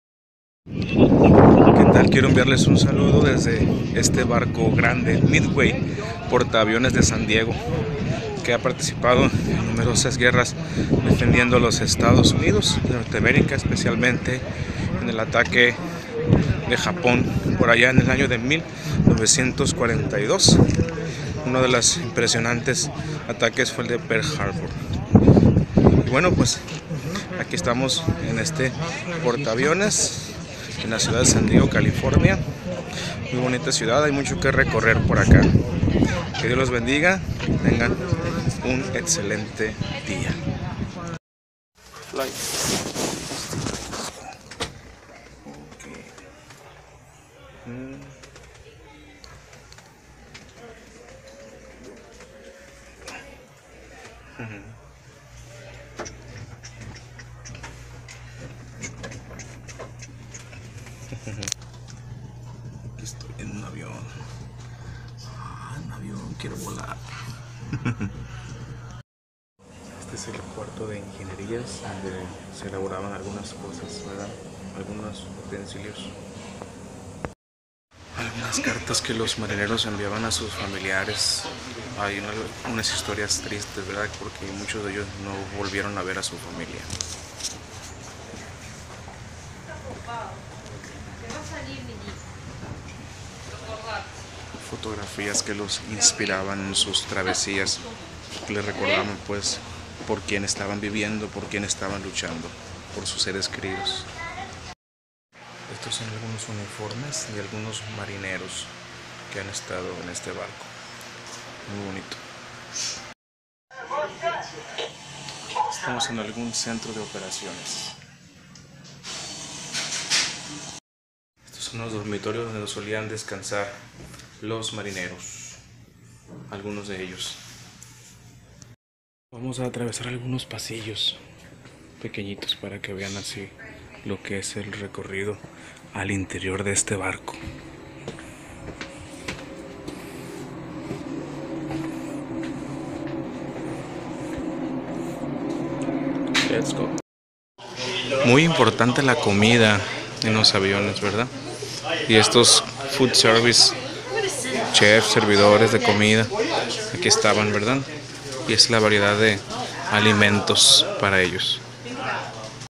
MIDWAY MUSEUM, SAN DIEGO, CAL.
Les comparto un poco de mi experiencia en el museo-barco porta aviones